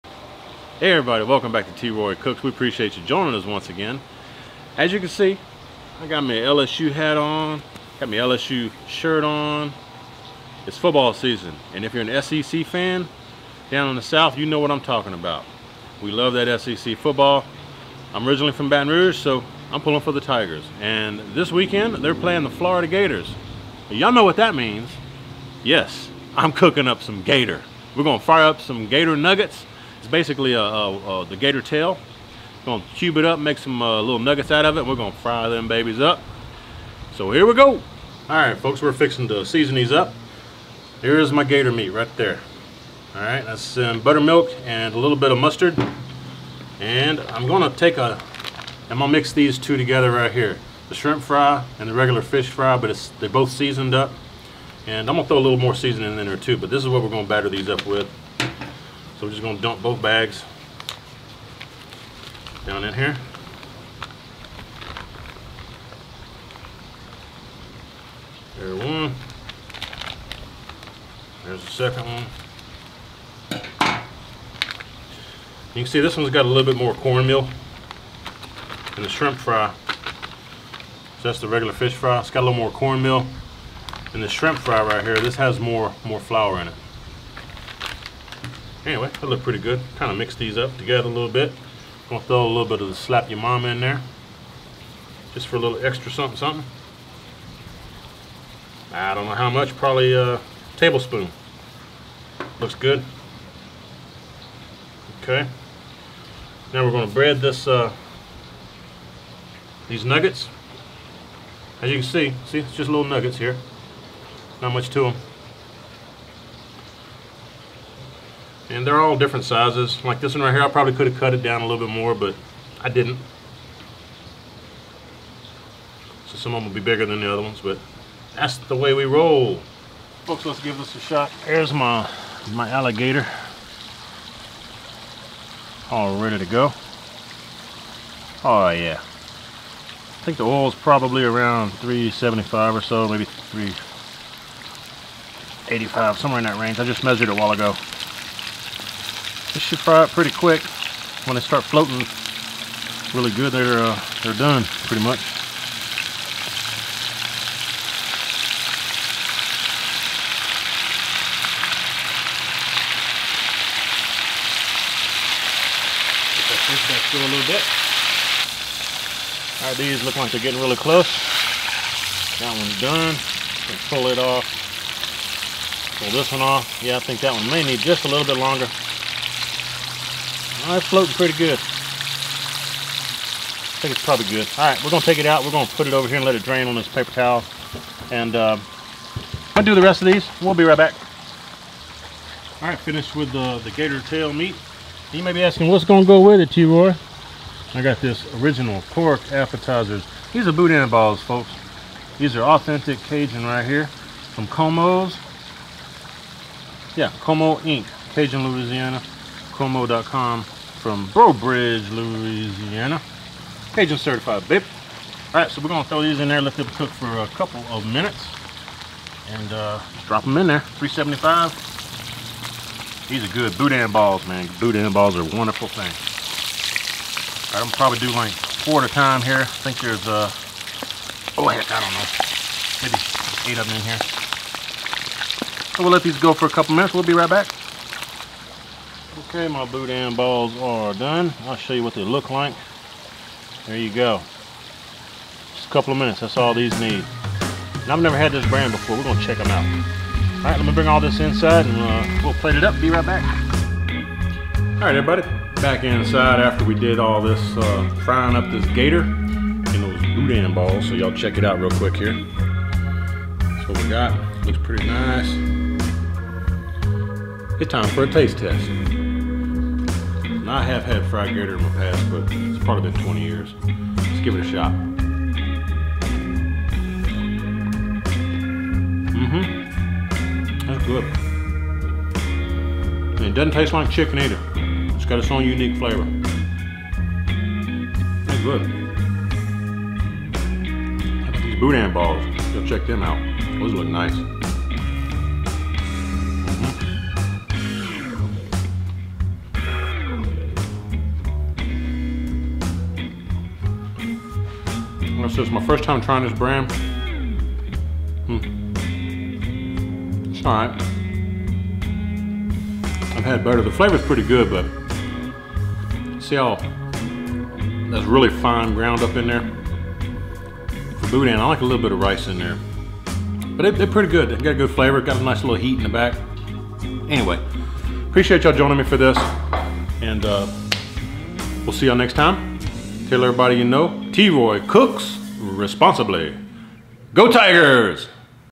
Cajun Fried Gator Nuggets and Boudin Balls
Deep fried Cajun Gator Nuggets and Boudin balls on the side. Life is good!!!

Canon G30 used to shoot this video: